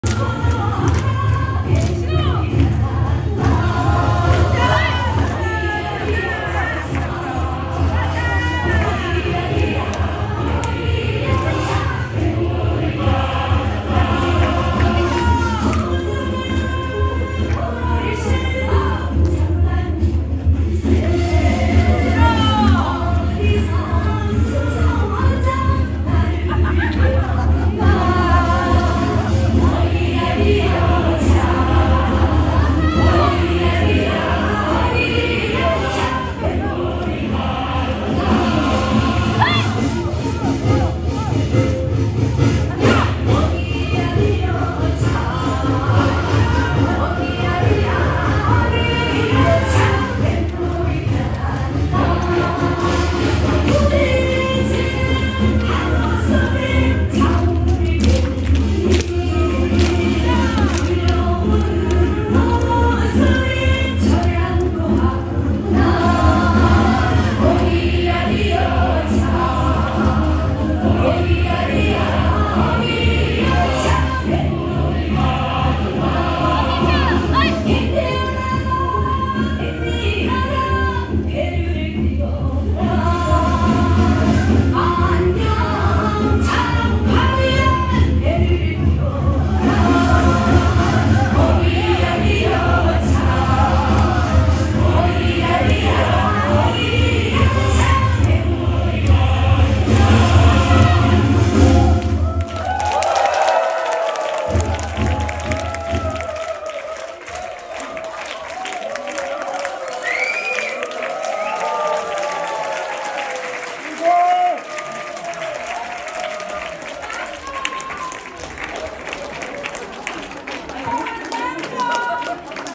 10a AOK de Esperanto - Korea Nacia Vespero 9/9 | 에스페란토 한국문화의 밤
10a Azia-Oceania Kongreso de Esperanto - la lasta kulmino de la Nacia Vespero
Nacia Vespero 9 - 10a Azia-Oceania Kongreso en Busano | 에스페란토 대회 한국의 밤